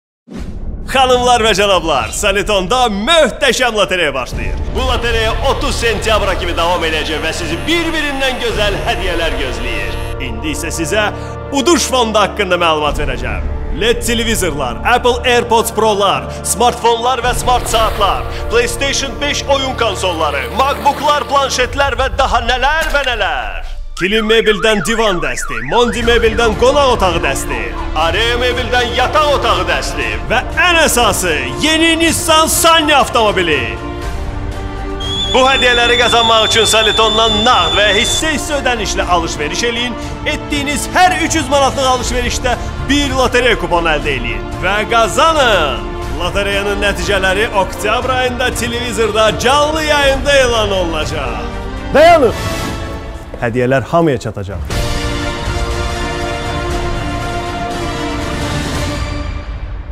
Soliton-dan Möhtəşəm Uduş kampaniyası!
Soliton-da Möhtəşəm Uduş kampaniyasına start verildi!
30 sentyabradək Soliton mağazalarından nağd və ya hissə-hissə ödənişlə edəcəyiniz hər 300 AZN-lik alış-verişdə 1 kupon qazanacaqsınız.

Uduş fondu:
LED televizorlar, Apple Airpods Pro və smart saatlar,Playstation 5 oyun konsolları, smartfonlar, planşetlər, macbooklar.

Kilim mebeldən divan dəsti
Mondi mebeldən qonaq otağı dəsti
Area mebeldən yataq otağı dəsti
Və əsas uduş, ən son Nissan Sunny avtomobili

Telefon 180